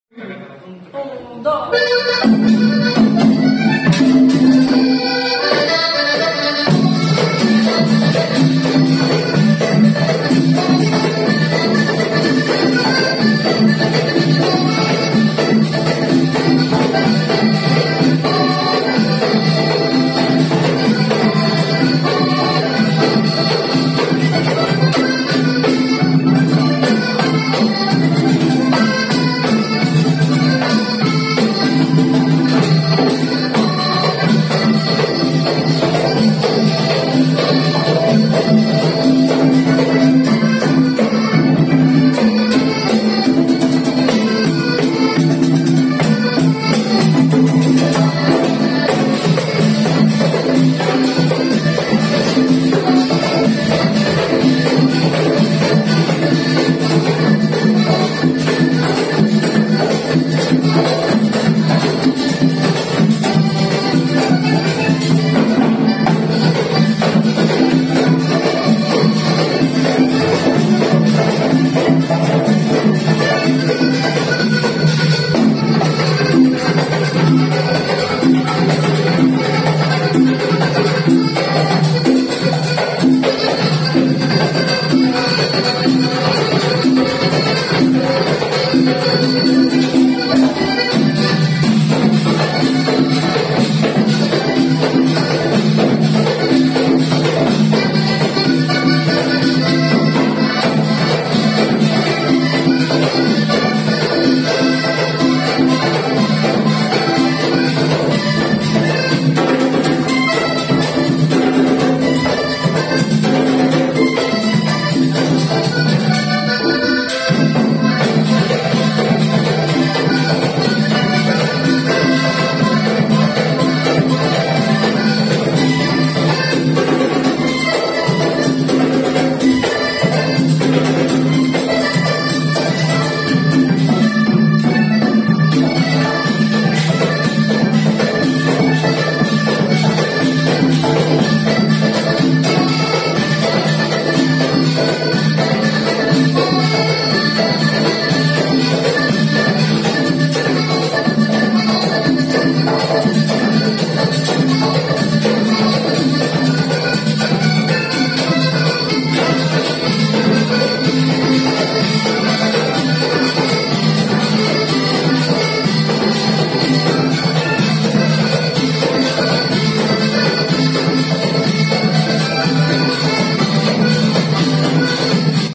Escuela de formación Vallenata de Manati. Dir Guillermo Lara Arrieta.
Escuela de formación musical VALLENATA  Manati Atlantico
Casa De La Cultura Mahabana Manati  , Gracias Dr. Evaristo Olivero Sanjuanelo  , Coordinador Jose Del Castillo   por tan importante apoyo. Instructores: Camila Mercado  Eddie Ceveriche, Lois Alberth Ortiz Caicedo  Dir  Guillermo Lara Arrieta  Guillermo Enrique Lara Arrieta